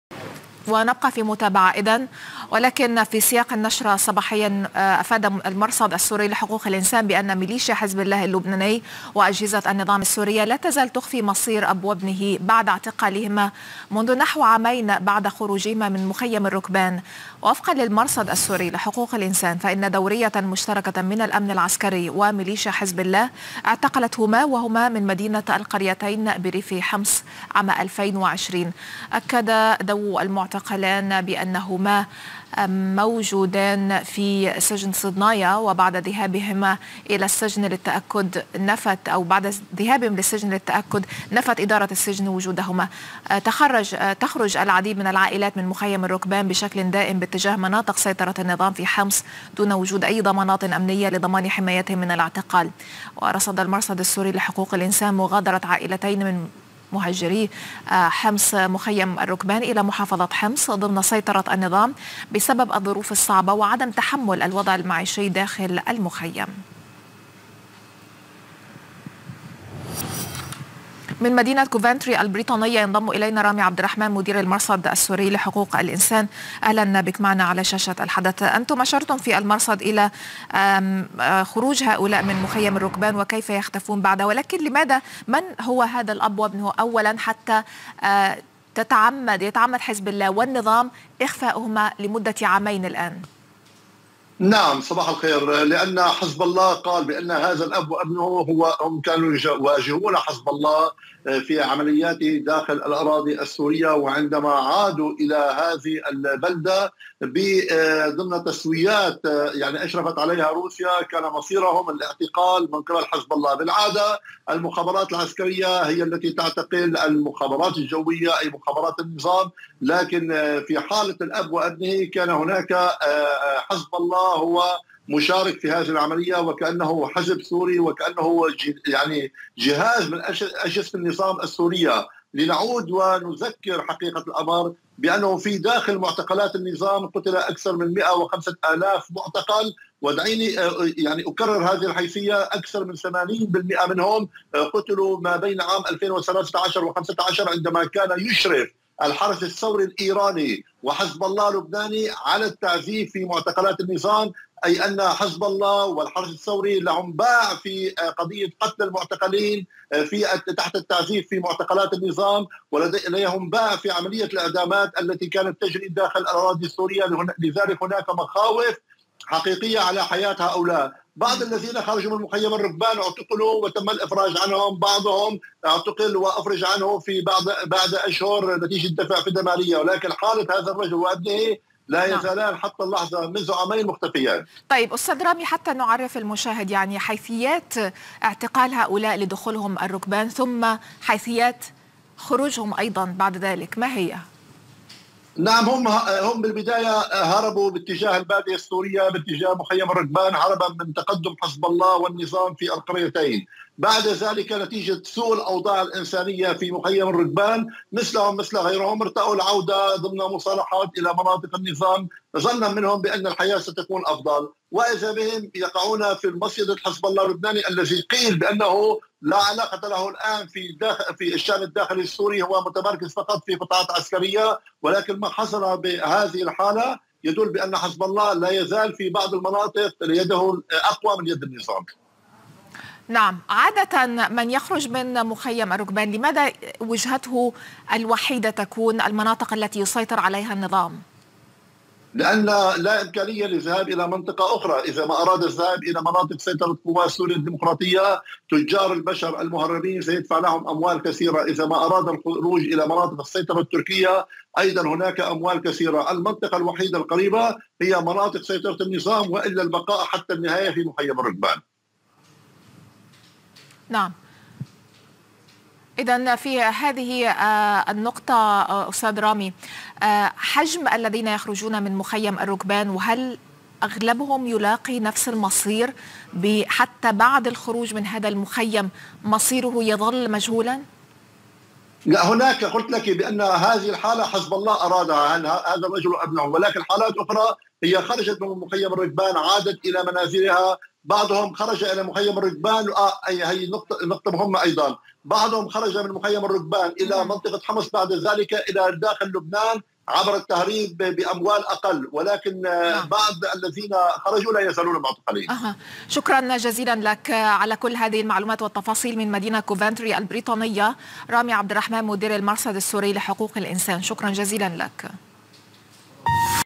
مدير المرصد السوري: داخل معتقلات النظام قُتل أكثر من 105 آلاف سوري
مدير المرصد السوري: داخل معتقلات النظام قُتل أكثر من 105 آلاف سوري، 80 بالمئة منهم قتل ما بين عامي 2013 و2015 عندما كان "الحرس الثوري الإيراني" و"حزب الله" اللبناني مشرفين على التعذيب، وبعض الذين خرجوا من مخيم الركبان اعتقلوا ومن ثم أخرجوا لقاء فدية مالية  إلا في حالة الأب وابنه فحزب الله هو من اعتقلهم قبل عامين وحتى الآن.
"حزب الله" يتصرف وكأنه حزب سوري وجهاز من أجهزة النظام، وهو لديه باع في عملية الإعدامات التي تحصل، ويده في بعض المناطق طائلة أكثر من يد النظام ذاتها.
المنطقة الوحيدة للخروج من مخيم الركبان هي مناطق النظام، لأن الذهاب إلى مناطق السيطرة التركية ستكلف أموال كثيرة باهظة، وأما مناطق سيطرة قوات سوريا الديمقراطية، فتجار البشر سيأخذون أموالا طائلة لقاء دخولهم إلى المنطقة.